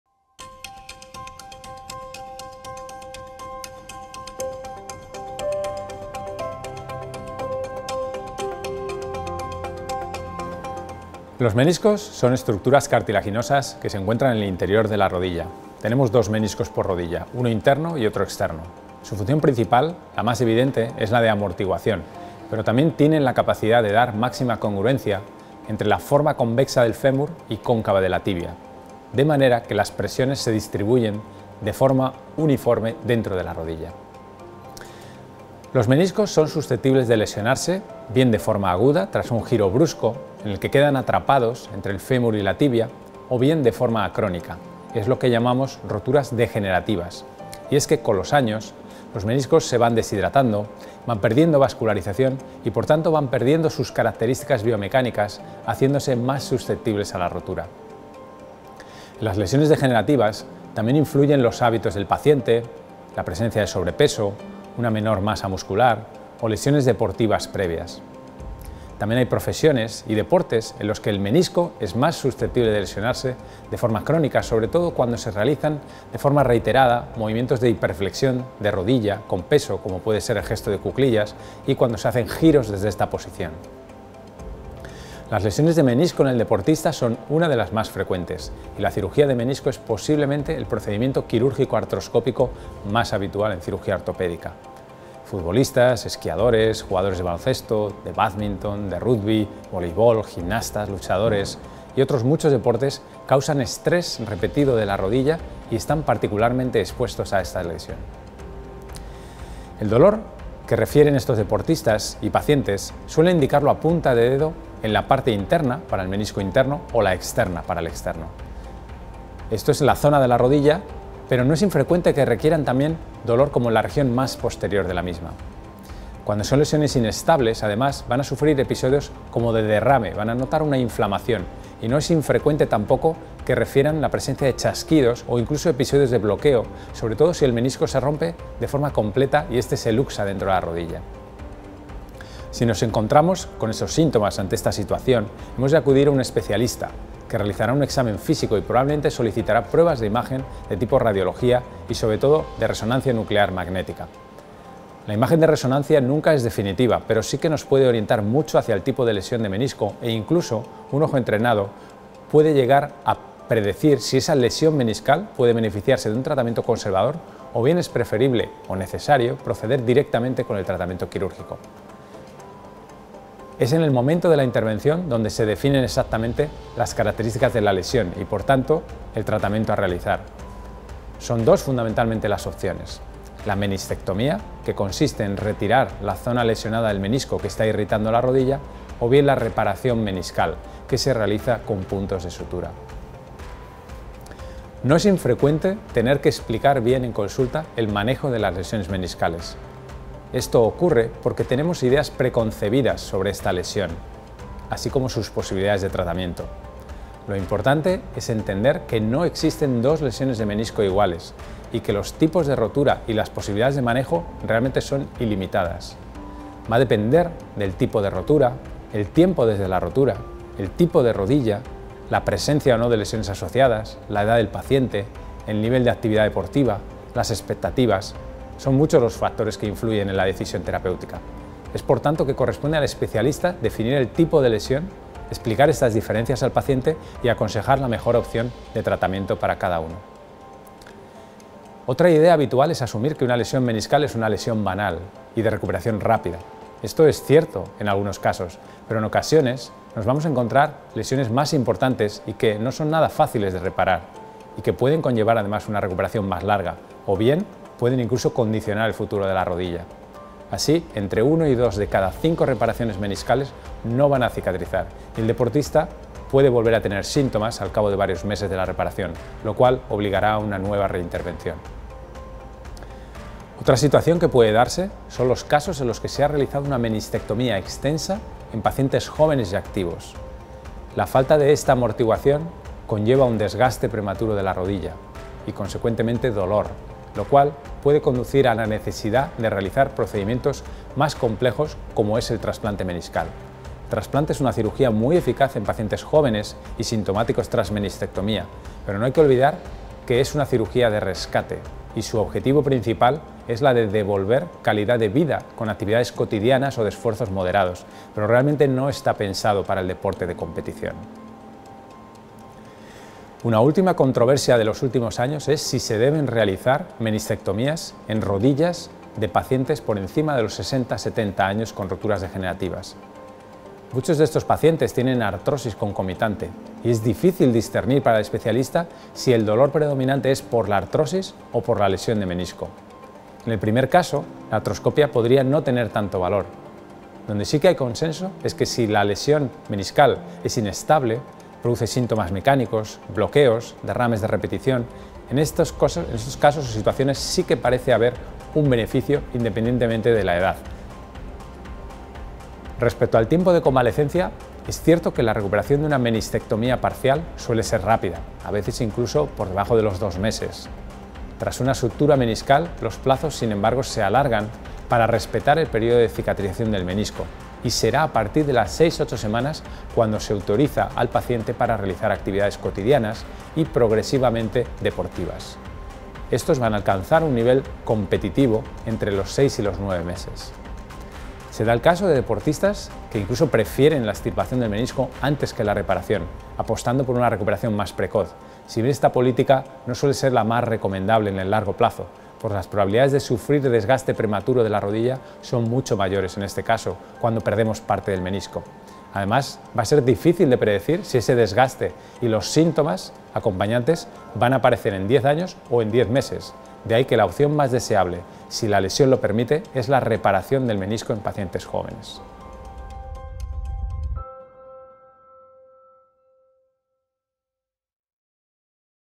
Todo lo que tenemos que saber sobre las lesiones de menisco - Dr. Samitier
Los meniscos son susceptibles de lesionarse, de forma aguda, tras un giro brusco en el que quedan atrapados entre fémur y tibia o bien de forma crónica, lo que llamamos roturas degenerativas y es que, con los años, los meniscos se van deshidratando y perdiendo vascularización y, por tanto, perdiendo sus características biomecánicas, haciéndose más susceptible a la rotura. En las lesiones degenerativas también influyen los hábitos del paciente, el sobrepeso, una menor masa muscular o lesiones deportivas previas. También hay profesiones y deportes en los que el menisco es más susceptible de lesionarse de forma crónica sobre todo cuando se realizan de forma reiterada movimientos de hiperflexión de rodilla y giros.

El Dr. Gonzalo Samitier, Jefe de Servicio de Traumatología y Cirugía Ortopédica te cuenta todo lo que debes saber sobre las lesiones de menisco.